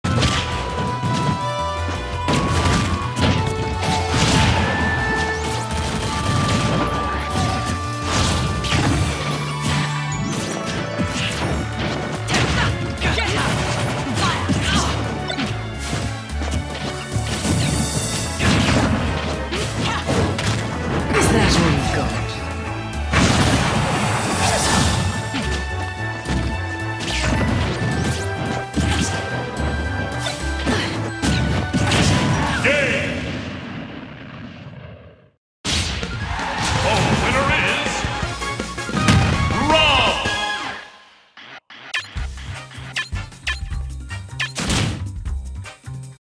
Don't fuck up your Witch Times
You might not get a second chance...

Nothing really spectacular here, I just found it amusing that after a Bayonetta fucked up their chance to kill me after a successful Witch Time, I managed to immediately kill them with my new favorite R.O.B. KO setup.